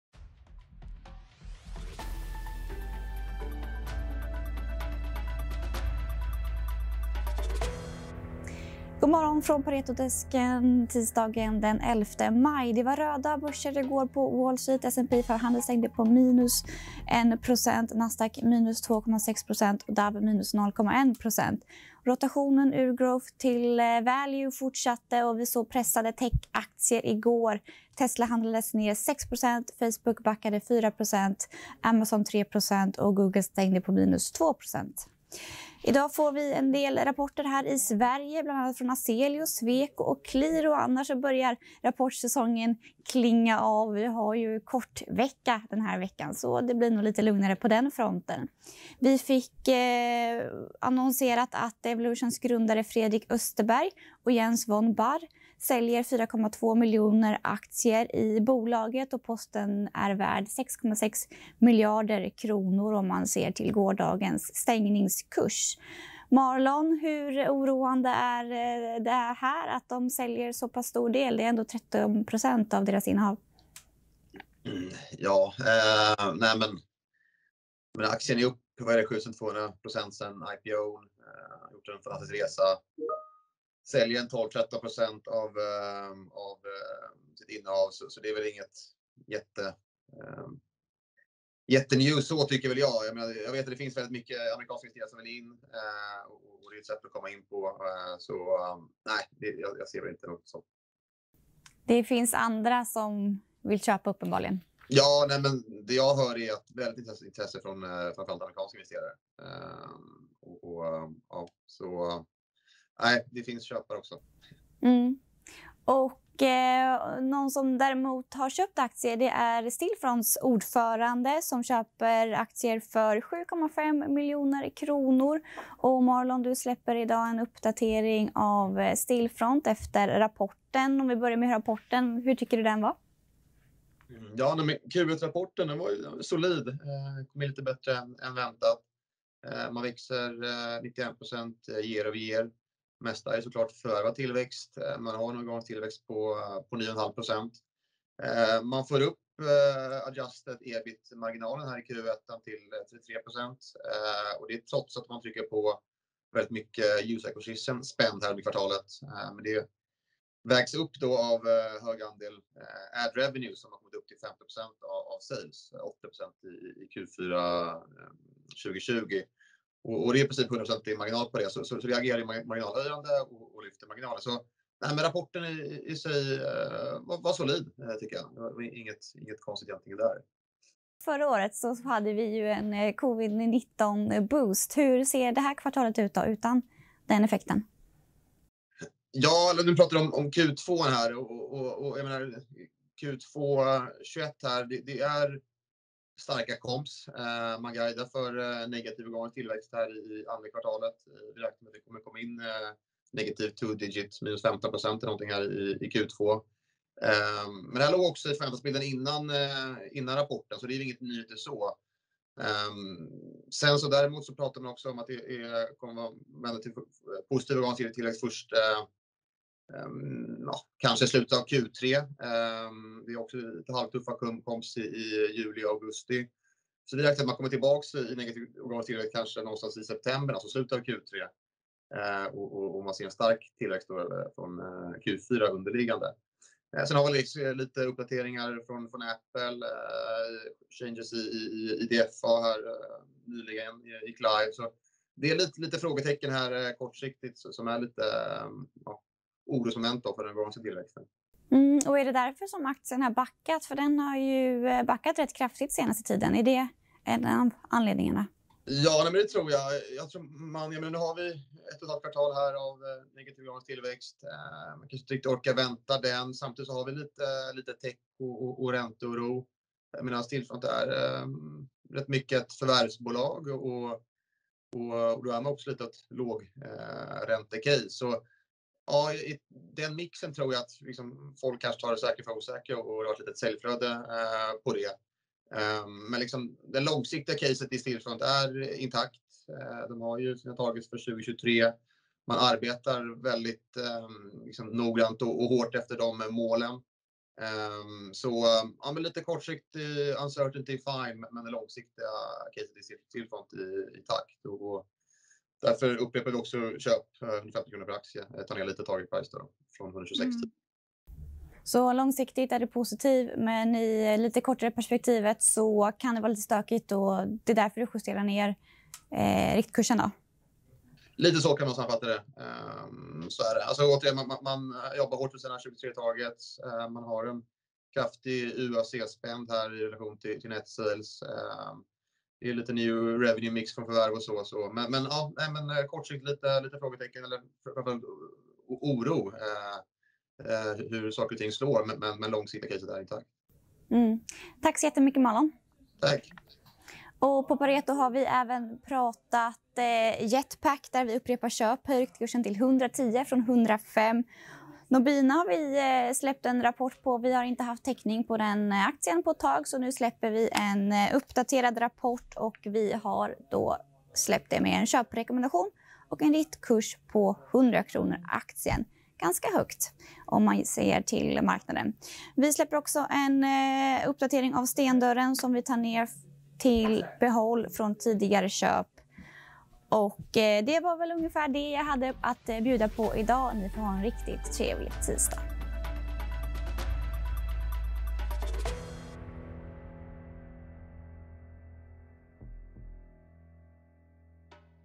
"Långsiktigt är Stillfront-caset intakt" / God morgon från Paretodesken 11 maj
Idag pratar vi om Stillfronts rapport och skakiga kursutveckling med Marlon Värnik. Dessutom kommenterar Marlon grundarnas försäljning av aktier i Evolution.